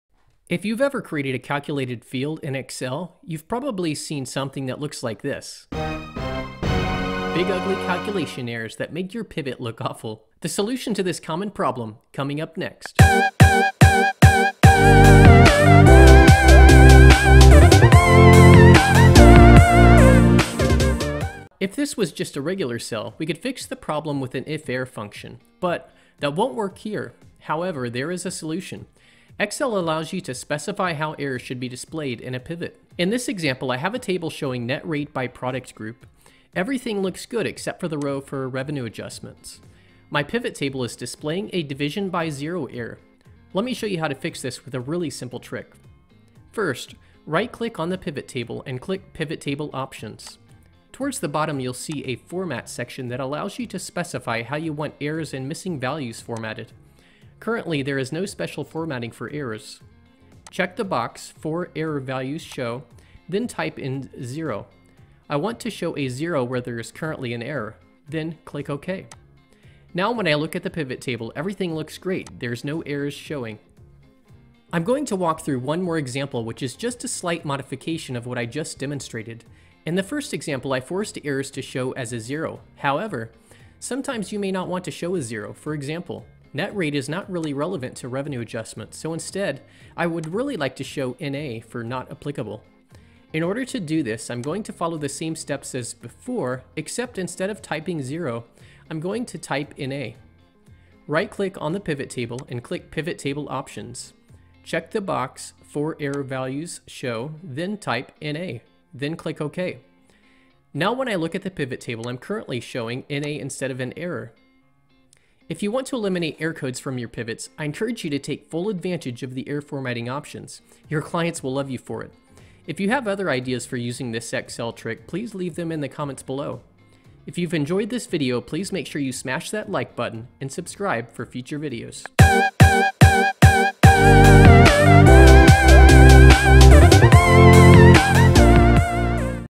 Pivot Table Errors in Excel | Remove or Replace Error Values in your Pivot Table
Excel Errors and Error values in Excel can be a pain. In this video, I demonstrate a show you a quick and easy way to hide Excel Errors in a Pivot Table. I don't mean mistakes... I mean formula errors such as DIV/0 or something like that. These kinds of errors are not a result of a mistake, but they DO make your pivot table look awful for presentation. So, with these simple tips and tricks, and this Excel Error Formula, error formatting is a breeze, and you can cleanup that pivot by hiding those unsightly error values in the pivot table through adjusting the pivot table error format settings.